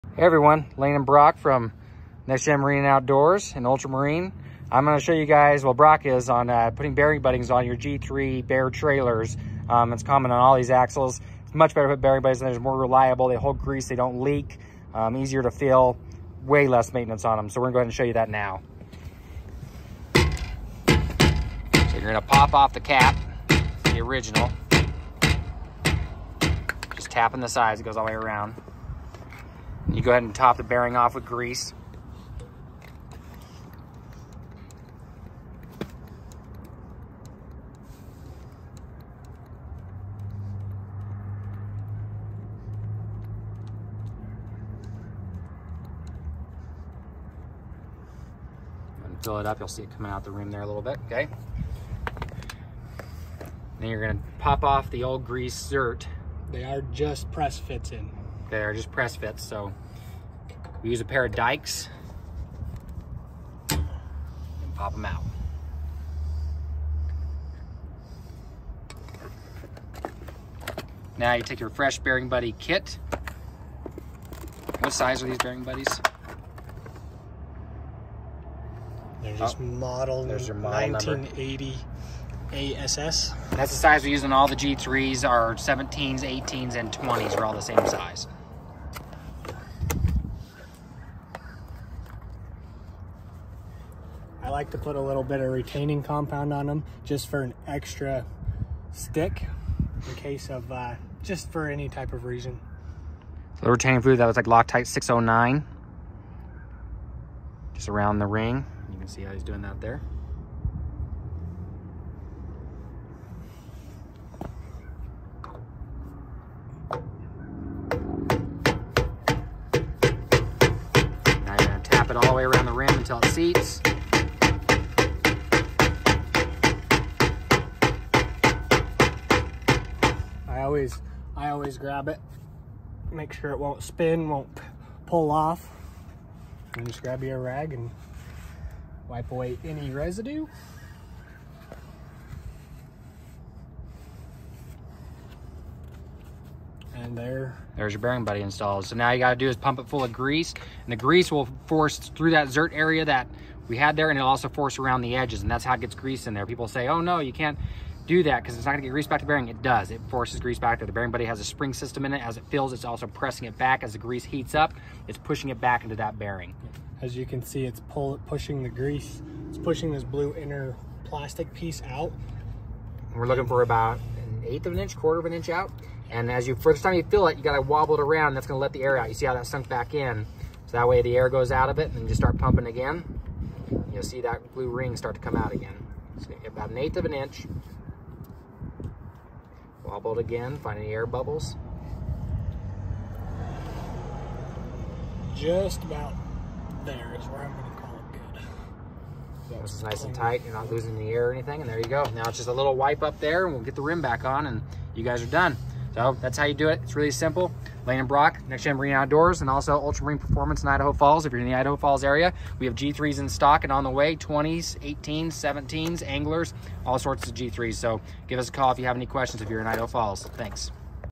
How To Install Bearing Buddy’s On Easy Lube Axles! G3 Boats.
Hey everyone! In the short video we are demonstrating how to install bearing buddies on your Bear trailers easy lube axles.

The easy lube desk covers tend to leak and fall off in transit. Bearing buddies at assurance of constant grease pressure and reliability with very low maintenance. I recommend this Be done to any trailer with this axle system.￼